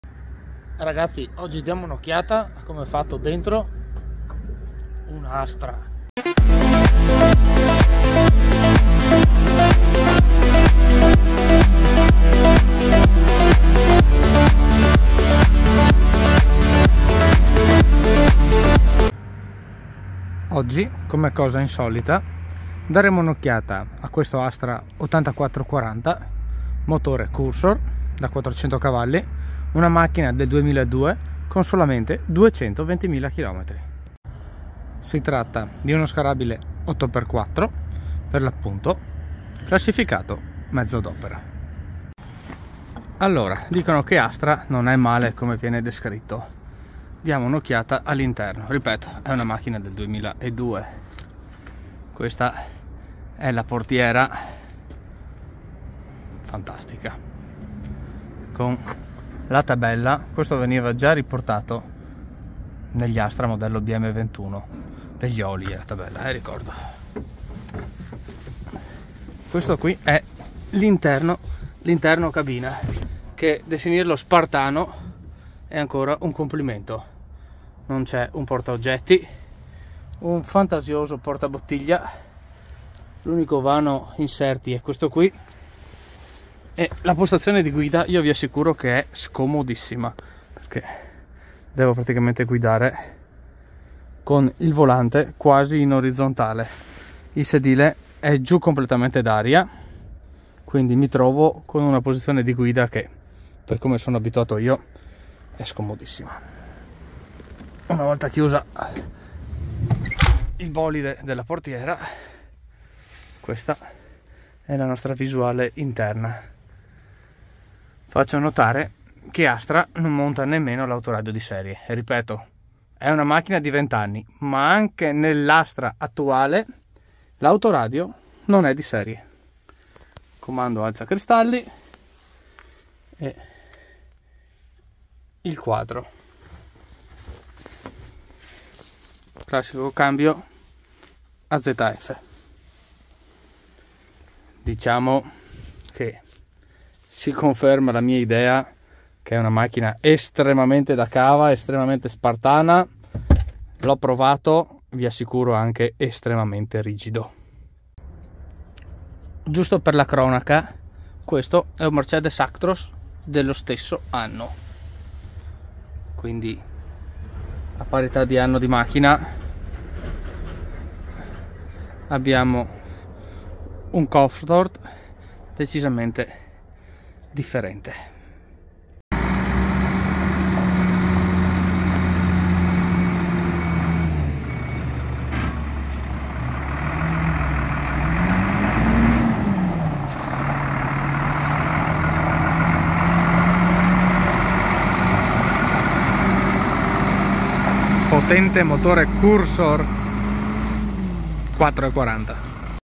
CAVA CANTIERE UN VERO DURO CAMION ASTRA 8440 SCOPRIAMOLO DA VICINO QUESTO MEZZO D'OPERA DA CANTIERE
PER ALTRE CURIOSITA O RICHIESTE BASTA CHE LO SCRIVIATE NEI COMMENTI QUI SOTTO E CERCHERO' DI ACCONTENTARVI!